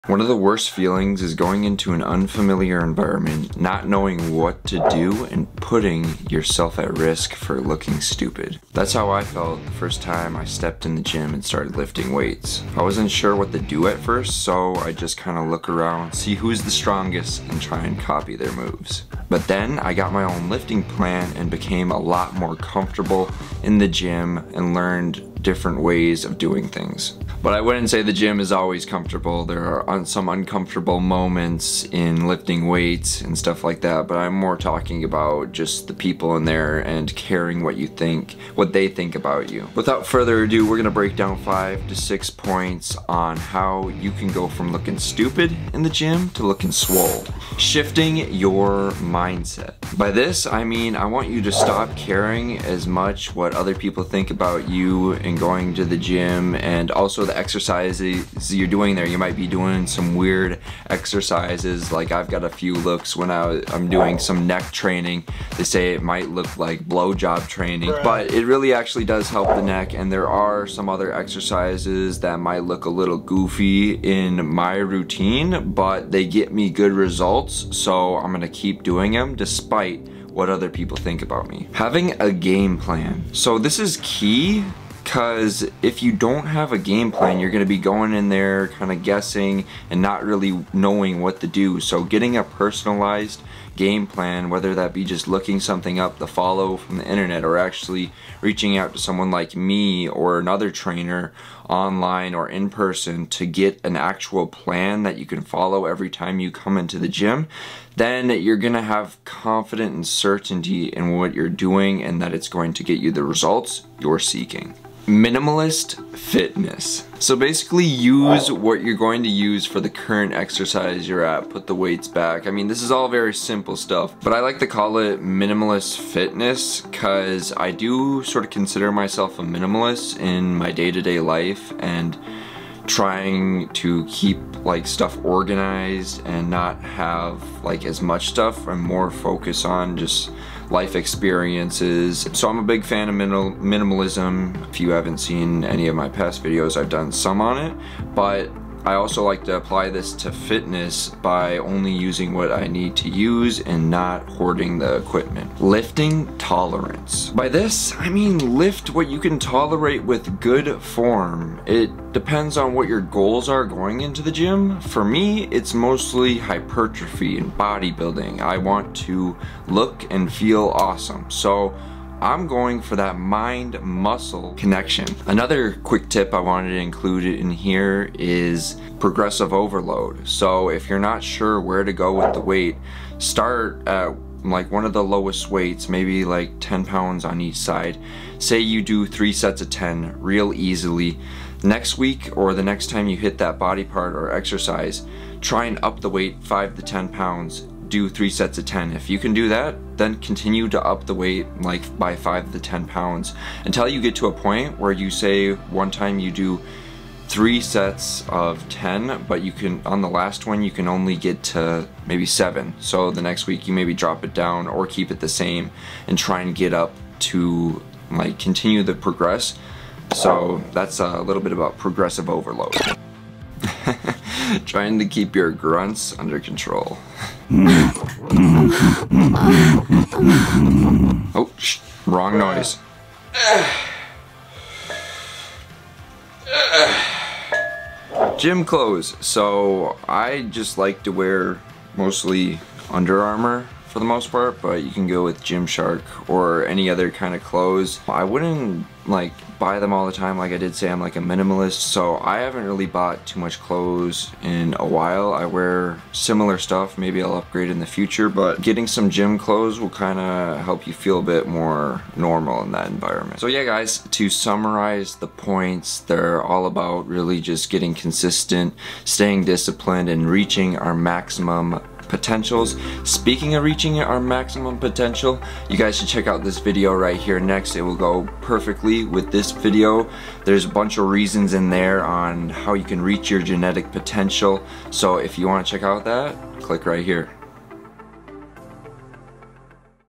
Afraid of Looking Stupid at the Gym? *Watch This*
In this video, we talk about you being scared of looking stupid at the gym. Along with other ways to not look stupid at the gym and get these gains. Overcoming gym anxiety 💪

Supplements I take:
Protein Powder
Green Powder